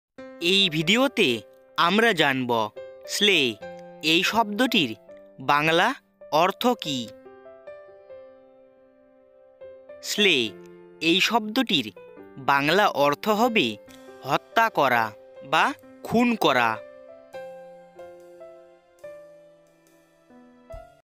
Slay Meaning in Bengali || Slay শব্দের বাংলা অর্থ কি? || Word Meaning Of Slay
এই ভিডিওতে slay শব্দের বাংলা অর্থ নিয়ে আলোচনা করা হয়েছে।

slay কে বাংলায় কি বলা হয়?
slay শব্দের বাংলা অর্থ কি?